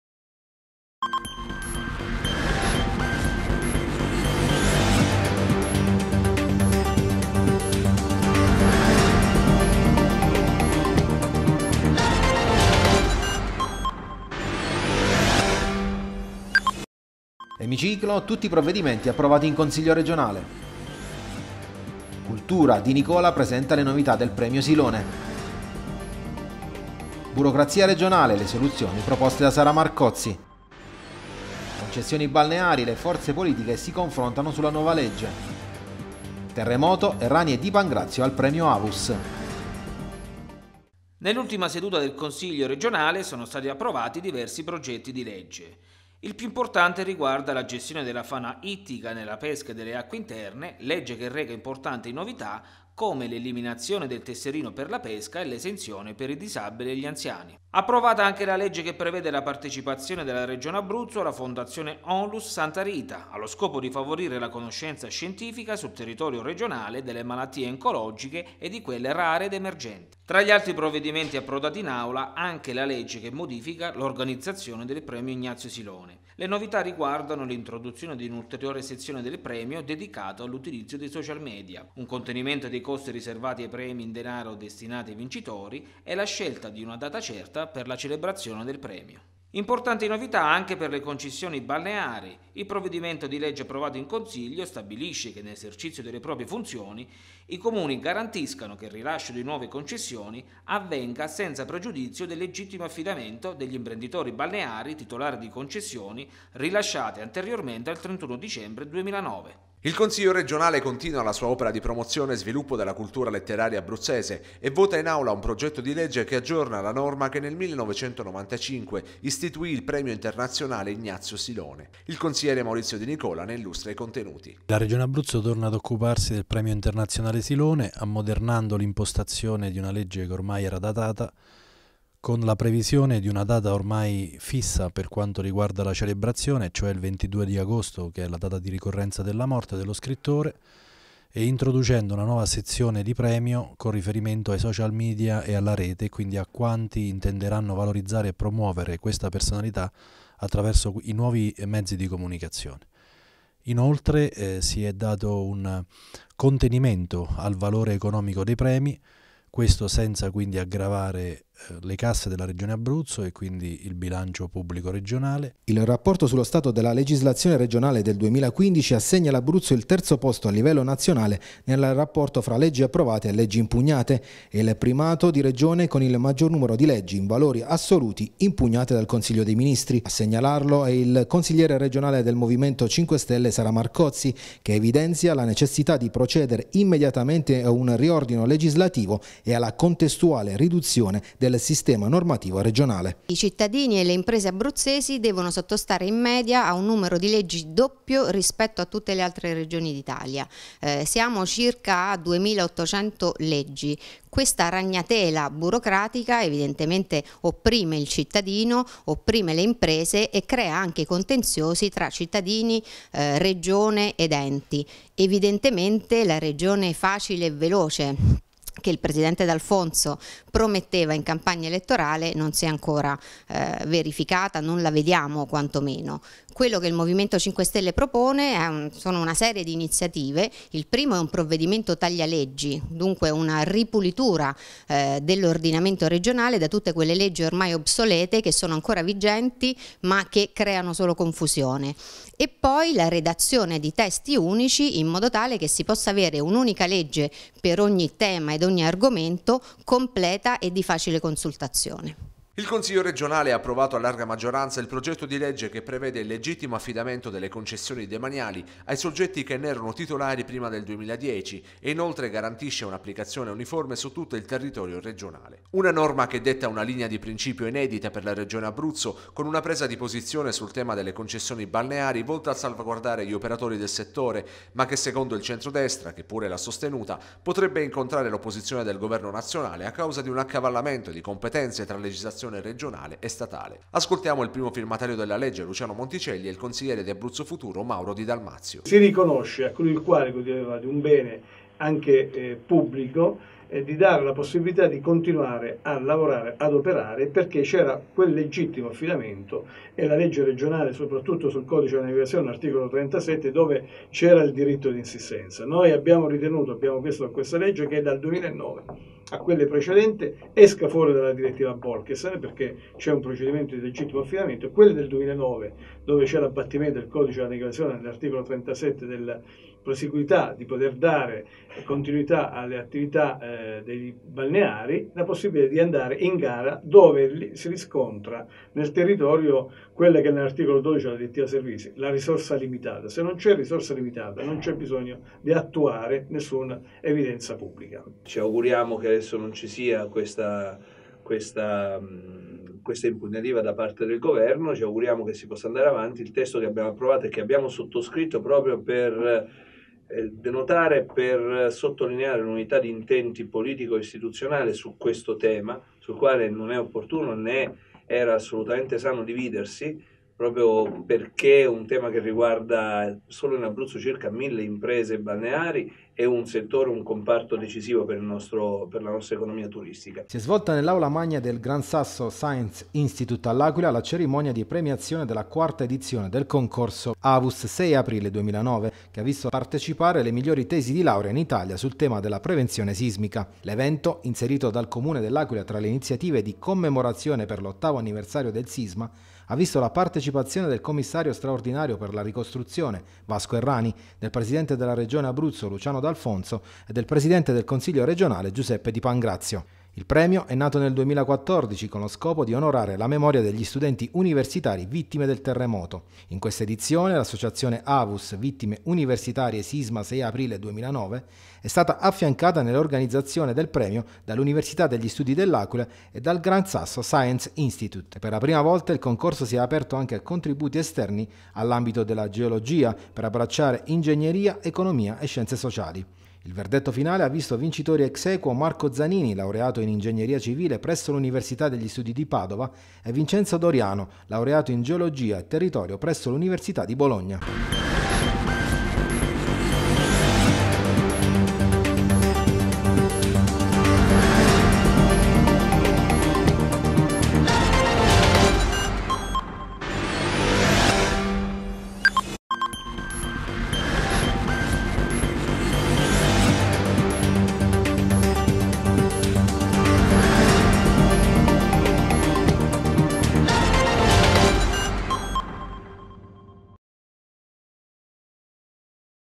TgWeb: Settimana 3-7 aprile
In questa edizione del TgWeb "InConsiglio": Emiciclo: tutti i provvedimenti approvati all'ultimo Consiglio regionale; Cultura: il Consigliere Maurizio Di Nicola presenta le novità del Premio Ignazio Silone; Concessioni balneari: le forze politiche si confrontano sulla nuova norma; Burocrazia regionale: le soluzioni del Consigliere Sara Marcozzi (M5S) per snellire i procedimenti; Vasco Errani all'Aquila per premiare le migliori tesi sulla sicurezza sismica.